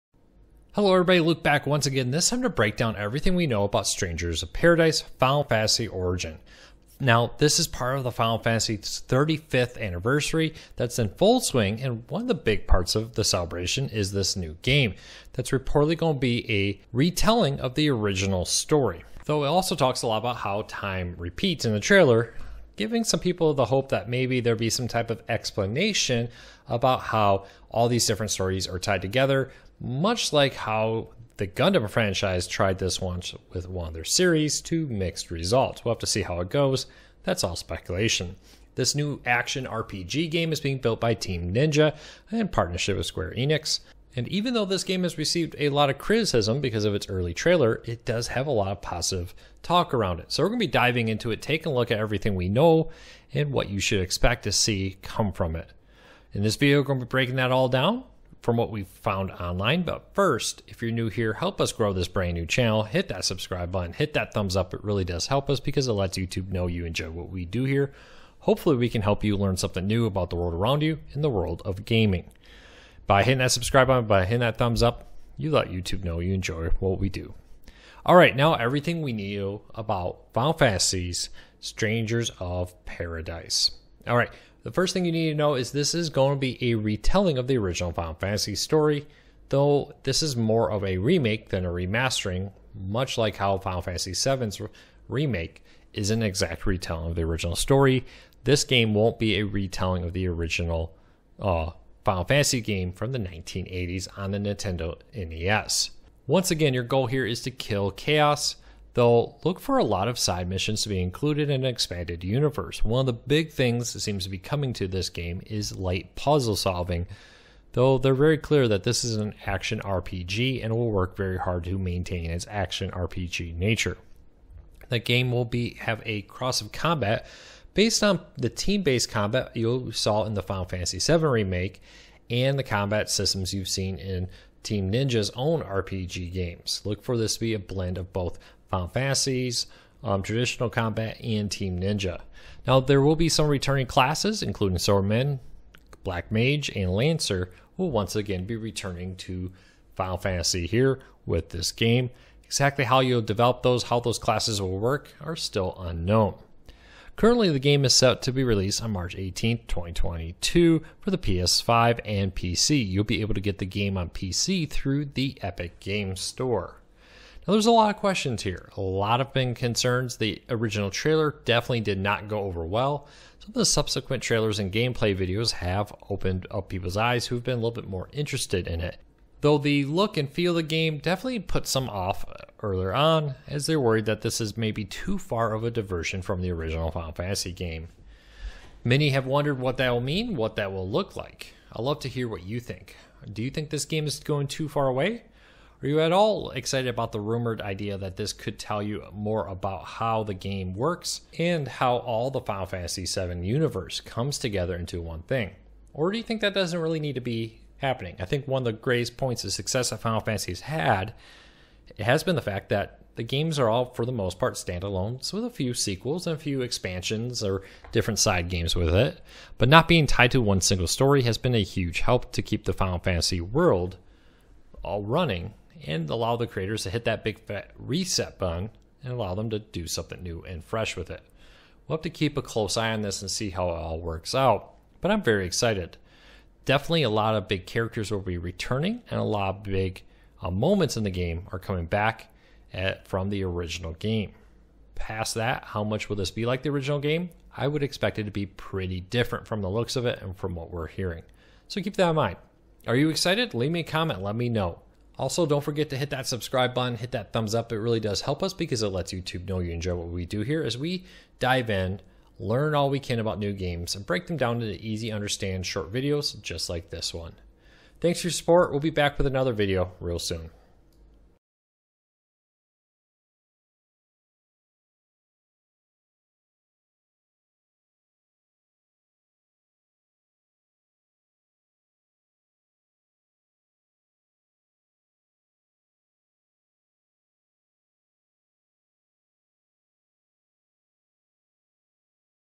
Everything We Know About Stranger of Paradise: Final Fantasy Origin – Release Date, Story, & More
Today we are going to break down everything we know about Stranger of Paradise: Final Fantasy Origin including its release date, story, rumors, & more. We also take a look at a few very creditable reports and break down what is happening with development Stranger of Paradise: Final Fantasy Origin.